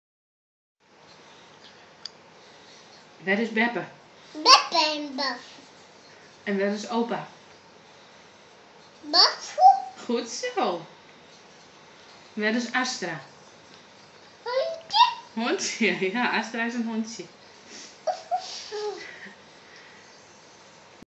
Tessa weet waar beppe en opa wonen ;-)
Inmiddels weet ze ook waar pake en oma wonen... Ze pikt steeds meer op en ze snapt je steeds beter. Nu wordt het echt oppassen geblazen met wat je zegt ;-)